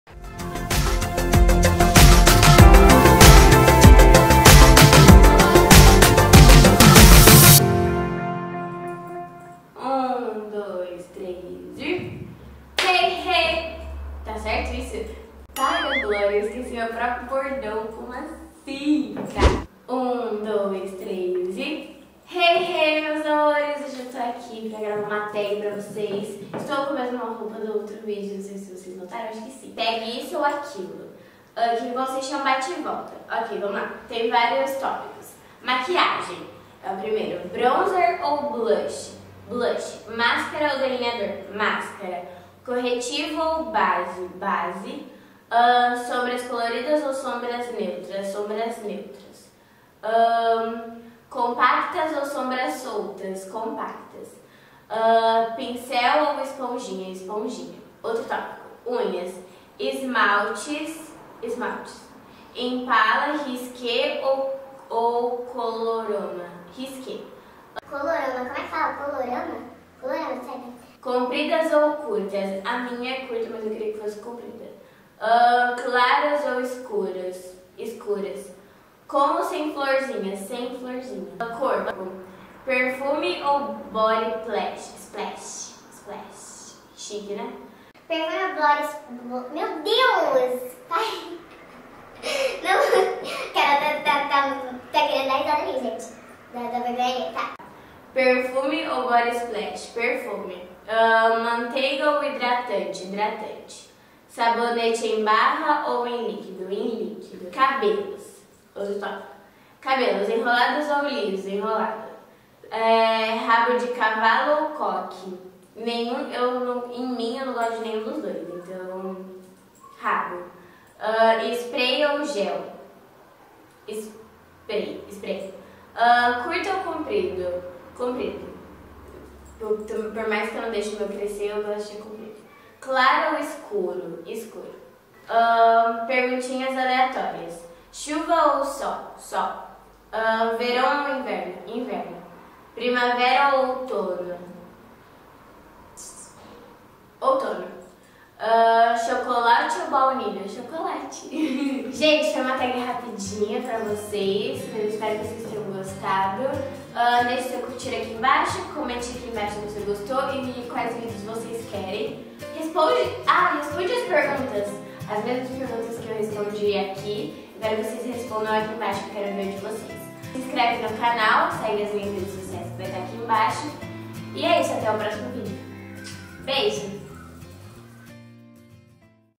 TAG - ISSO OU AQUILO !?
Perguntas:

Maquiagem
1. Bronzer ou blush?
2. Máscara ou delineador?
3. Corretivo ou base?
4. Sombras coloridas ou sombras neutras?
5. Compactas ou sombras soltas?
6. Pincel ou esponjinhas?

Unhas
1. Esmates : Impala, Risqué ou Colorama?
2. Compridas ou curtas?
3. Claros ou escuros?
4. Com ou sem florzinha?

Corpo
1.Perfume ou body splash?
2.Manteiga ou hidratante?
3. Sabonete em barra ou líquido?

Cabelos
1. Enrolados ou lisos?
2. Rabo de cavalo ou coque?
3. Spray ou gel?
4. Curto ou comprido?
5. Claro ou escuro?

Perguntinhas Aleatórias
1. Chuva ou sol?
2. Verão ou inverno?
3. Primavera ou outono?
4. Chocolate ou baunilha?